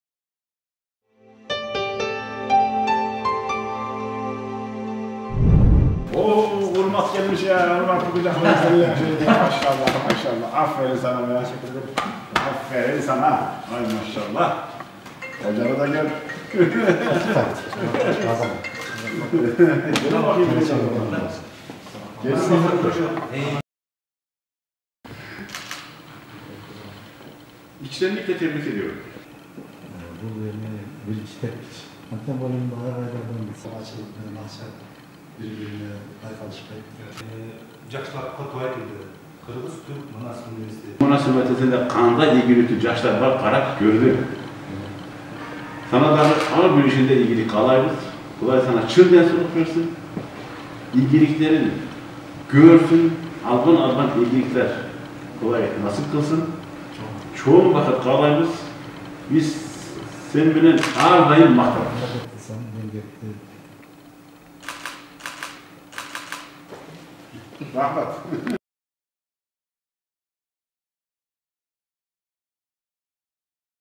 КТМУ Урмат Токомбаевди куттуктады
"Асман" долбоорунун жеңүүчүсү КТМУ студенти Урмат Токомбаевди университетибиздин жетекчилиги кабыл алды